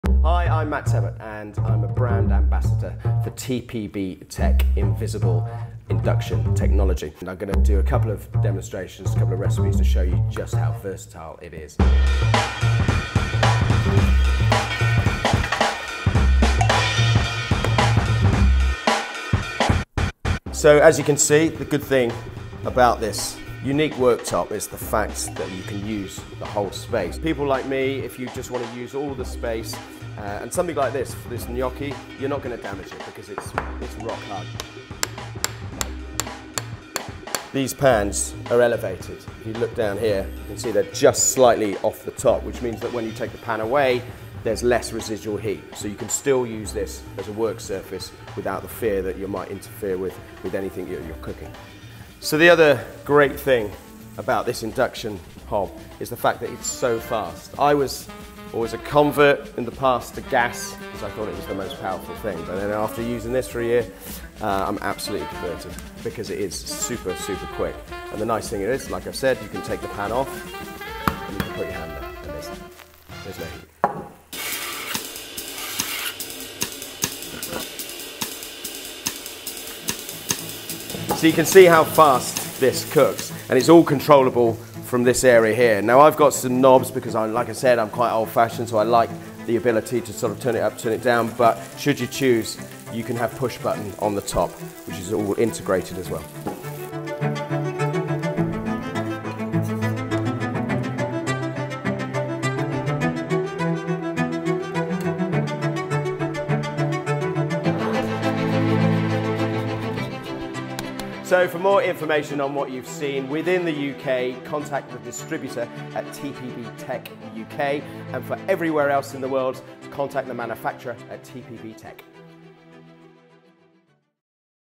Matt Tebbutt explains why TPB Tech invisible induction is the next level in kitchen appliances.
Matt Tebbutt, TPB Tech ambassador explains why TPB Tech appliances are the next level on kitchen appliances.

A multipurpose worktop, cook prep and plate up all within the same space. Visually appealing and incredibly versatile cooking worktop.

A truly unique surface for every modern home.

TPB Tech redefines the meaning of open plan kitchen/living space. A truly unique surface for every modern home.

 Tempted by the sound of an invisible induction worktop from TPB Tech?